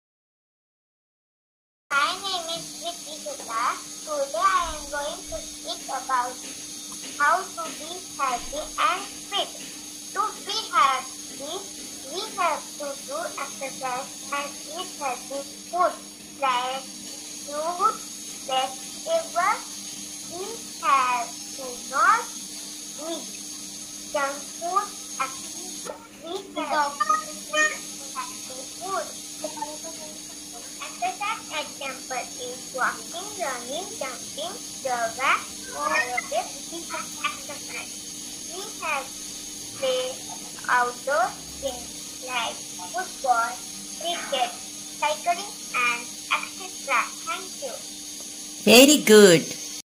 How to stay healthy and fit by Dwiti ( class 3#onlineclasses
How to stay healthy and fit by Dwiti ( class 3rd )in online spoken class